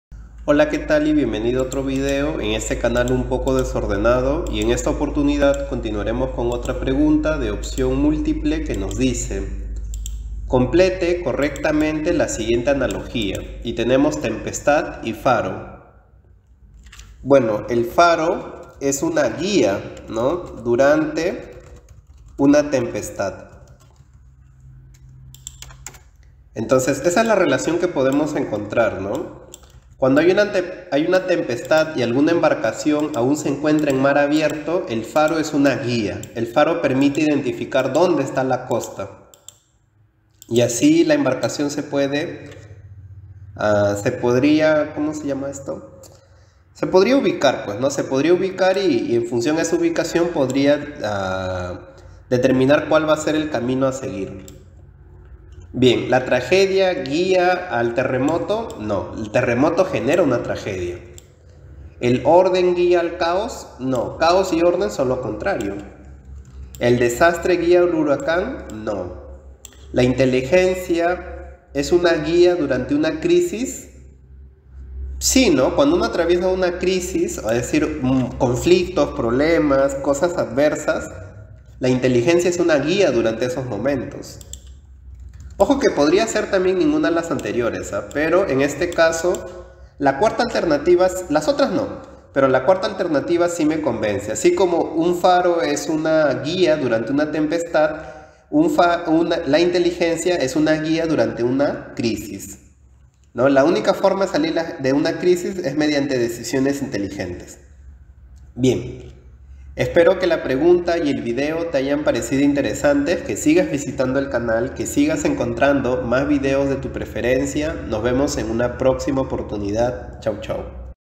1612 - Pregunta resuelta sobre analogías | Tempestad : Faro
OPCIÓN MÚLTIPLE
Complete correctamente la siguiente analogía:
Tempestad: Faro
◯ terremoto: tragedia
◯ caos: orden
◯ huracán: desastre
◯ crisis: inteligencia
◯ ninguna anterior